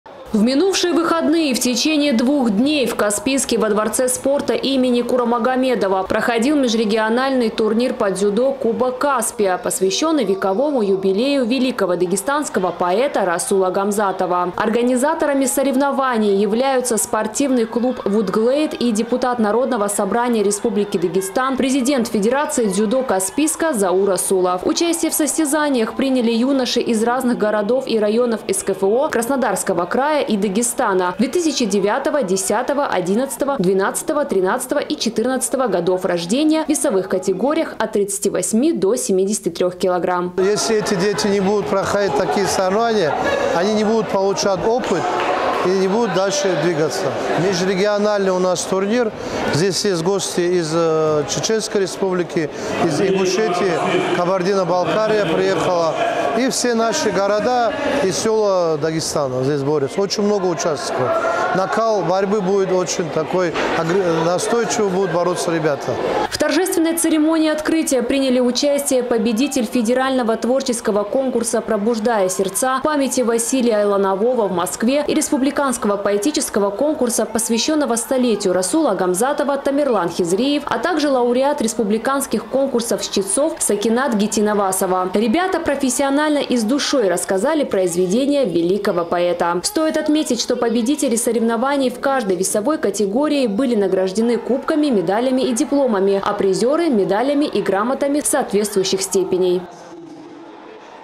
Межрегиональный турнир по дзюдо «Кубок Каспия» прошел во Дверце спорта им. Курамагомедова
В минувшие выходные в течении двух дней в Каспийске во Дворце спорта им. Курамагомедова проходил межрегиональный турнир по дзюдо «Кубок Каспия», посвящённый вековому юбилею великого дагестанского поэта Расула Гамзатова. Организаторами соревнований являются спортивный клуб Вудглейд и депутат Народного Собрания РД, президент Федерации дзюдо Каспийска Заур Расулов

Участие в состязаниях приняли юноши из разных городов и районов СКФО, Краснодарского края и Дагестана 2009, 2010, 2011, 2012, 2013 и 2014 годов рождения в весовых категориях от 38 до 73 кг.